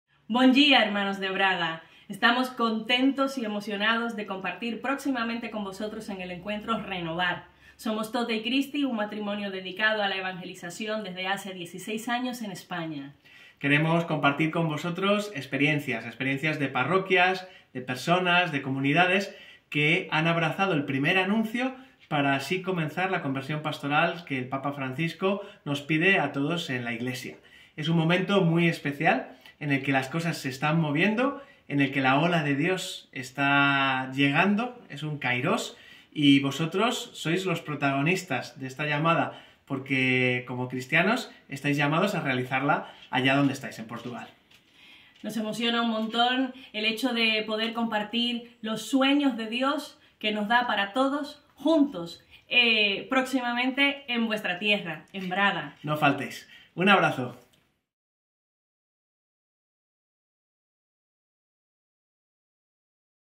Jornada Pastoral - RENOVAR - Convite Tote Barrera e Cristy Salcedo
Tote Barrera e Cristy Salcedo serão os conferencistas principais da “Jornada Pastoral - RENOVAR” e convidam para este momento de renovação espiritual, que acontece nos dias 19 e 20 de outubro, no Auditório Vita, em Braga. Já fez a sua inscrição?